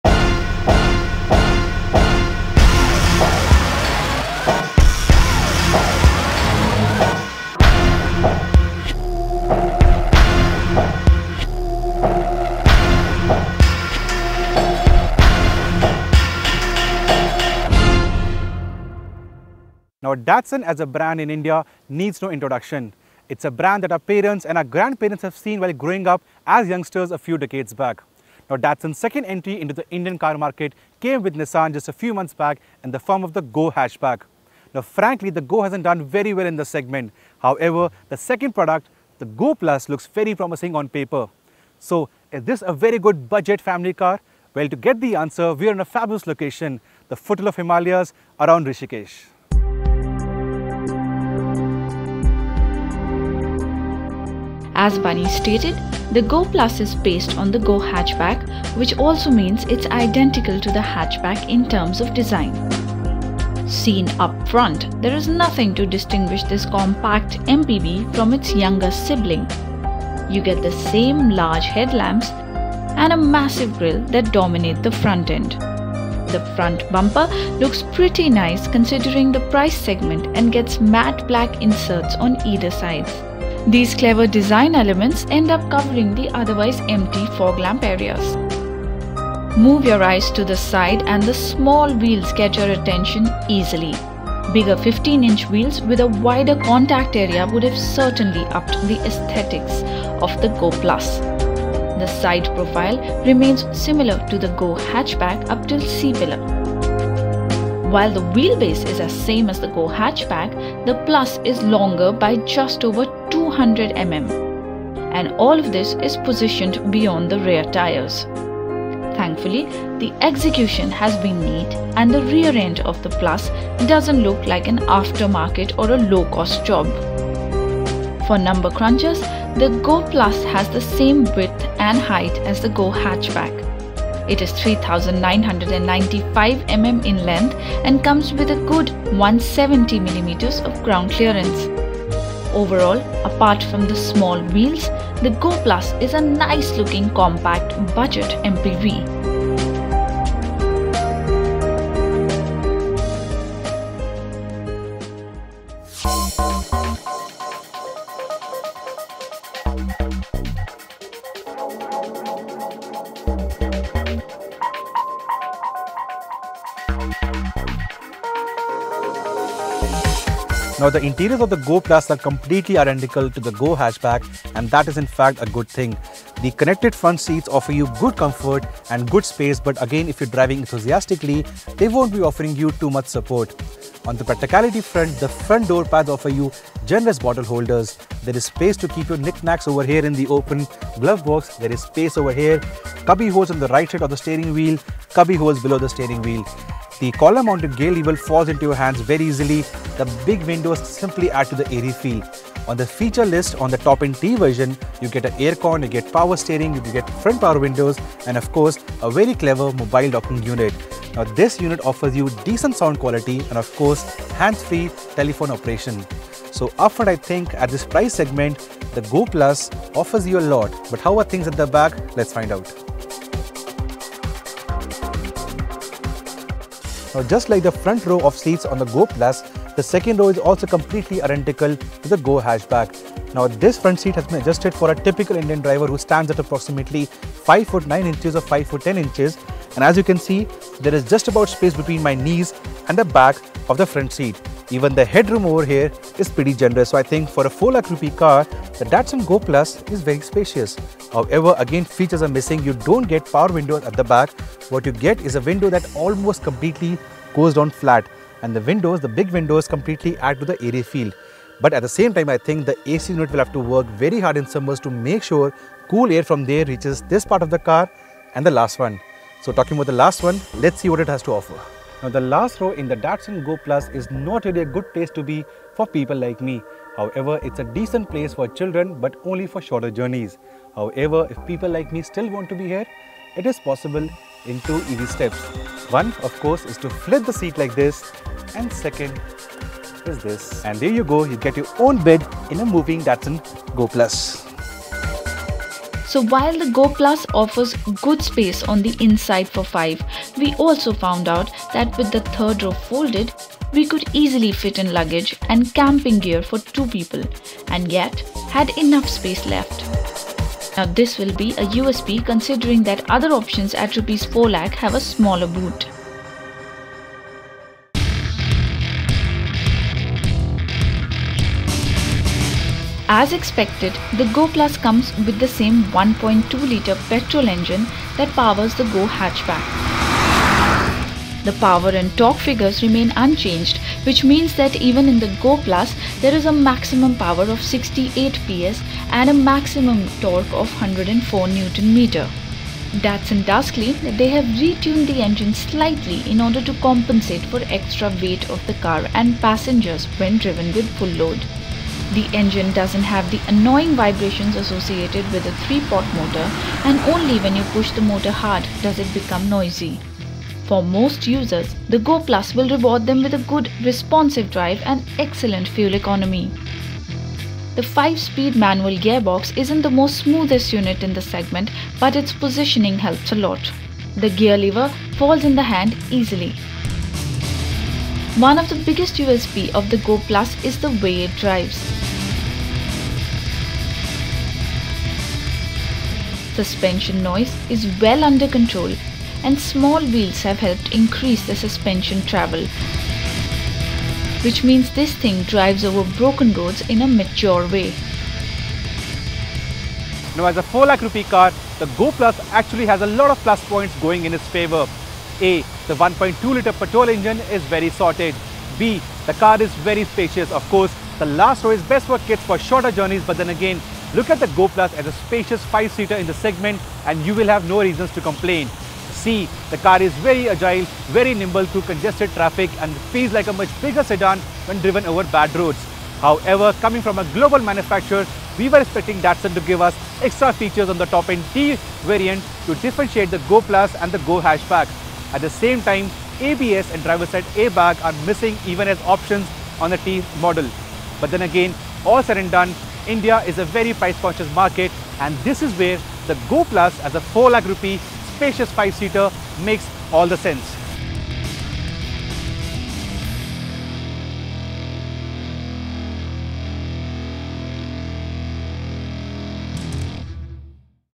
Datsun Go+ First Drive in India
India's first compact MPV, Datsun Go+ will be launched in January. We take it for spin to get to know the Go+ better. Here are our first impressions of this all new MPV.